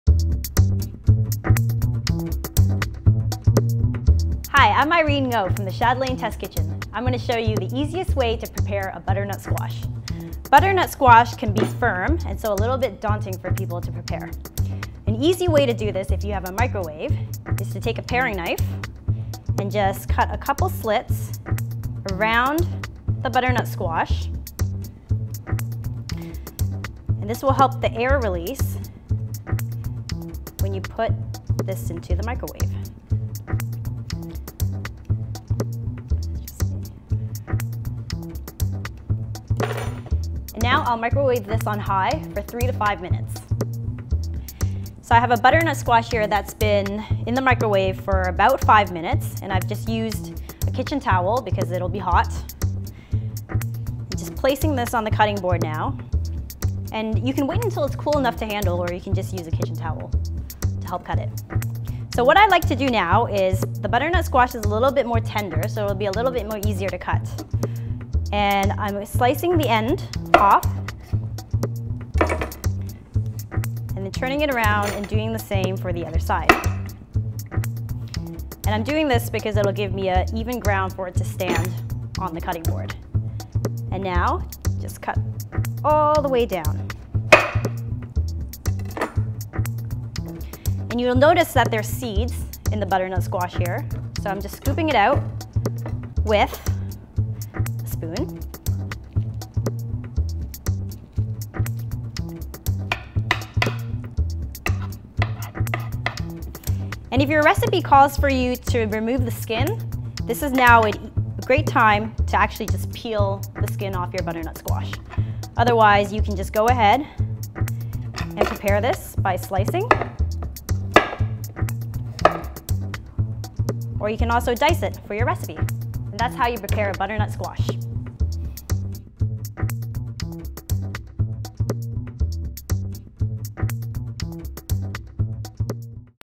A simple and safe way to cut butternut squash
How to prepare this root vegetable using a simple trick